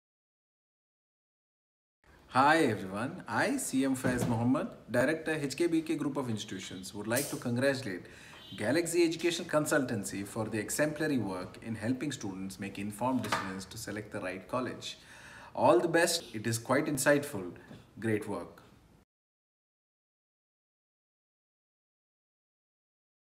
Appreciation from HKBK Group of Institutions
Director of HKBK Group of Institutions, Mr. C.M. Faiz Mohammed appreciating Galaxy Education for their excellence.